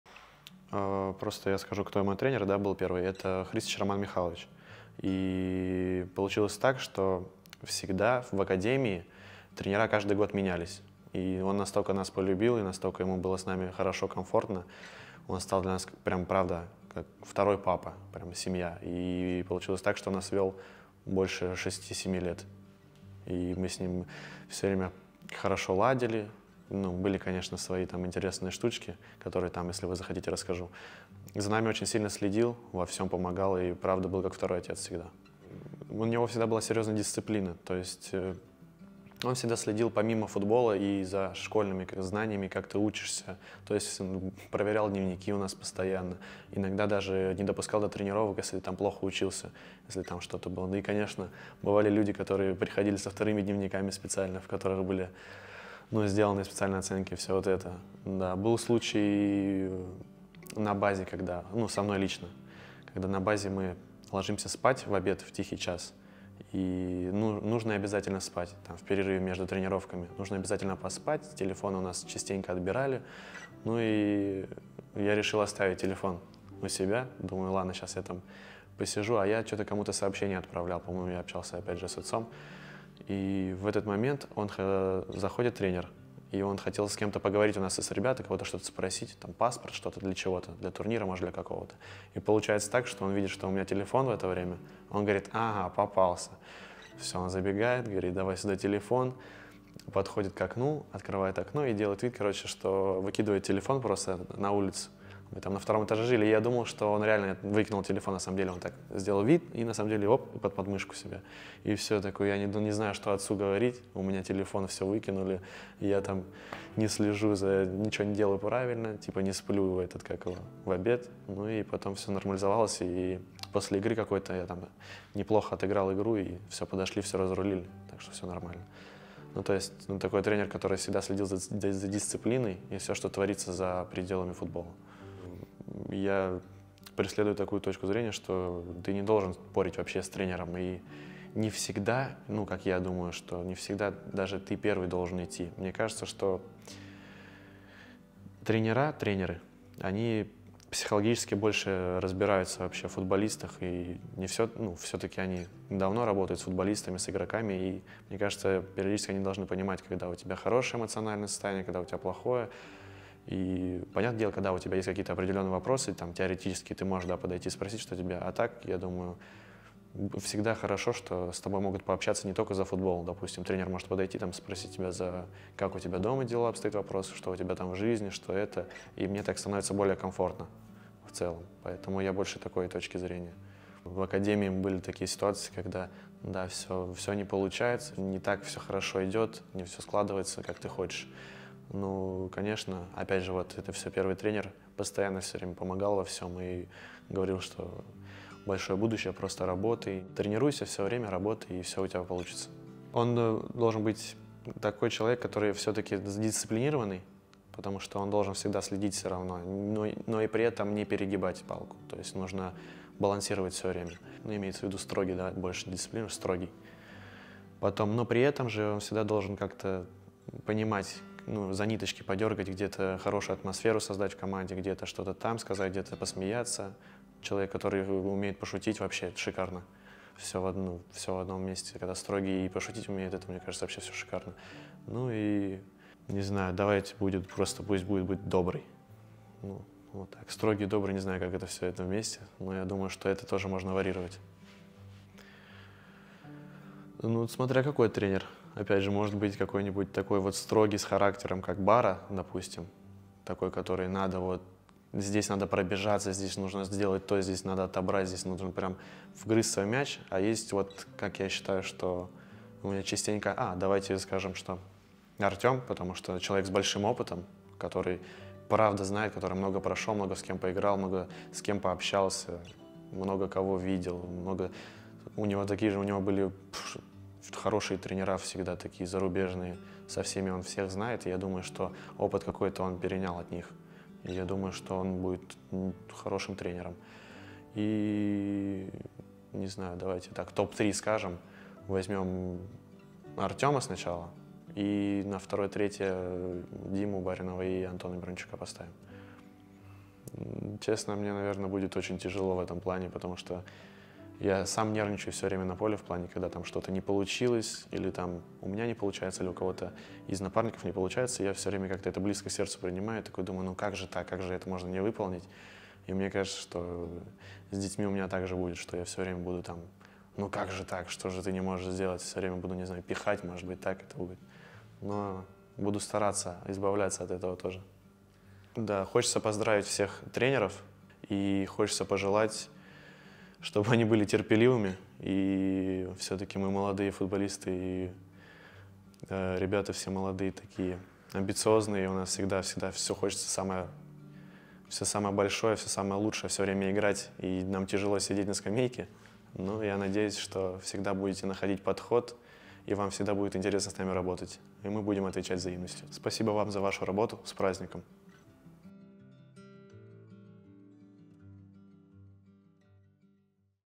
Константин Марадишвили: Тренерами могли бы стать Дзюба, Баринов и Миранчук
В честь сегодняшнего Дня тренера мы пообщались с Костей Марадишвили и задали интересные тематические вопросы 🤩

Мара поздравил всех с профессиональным праздником и рассказал:

👦 О своём первом тренере
🙌 О психологической поддержке и дисциплине
😱 О том, кто бы из нашей команды смог бы сам стать наставником

Включай новый ролик 😉